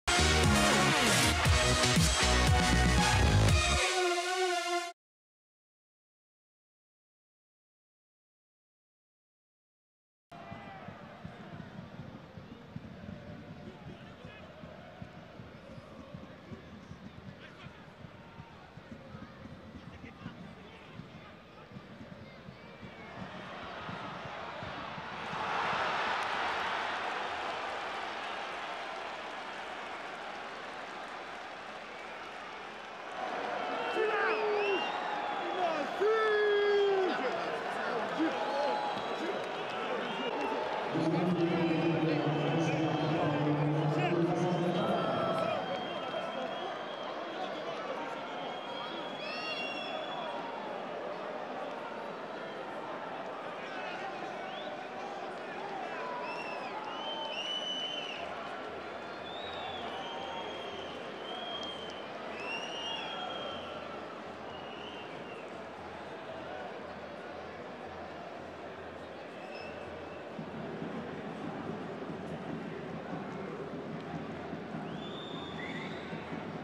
Goal Abdoul Razzagui CAMARA (34') / Toulouse FC - Angers SCO (1-2) - (TFC - SCO) / 2015-16
Toulouse FC vs Angers SCO (1 - 2) : Abdoul Razzagui CAMARA (34') goal. All Toulouse FC vs Angers SCO goals in video.

Stadium - Sat, 17 Oct 2015

Goals : Oscar TREJO (24' - Toulouse FC) / Cheikh NDOYE (27' - Angers SCO) / Abdoul Razzagui CAMARA (34' - Angers SCO)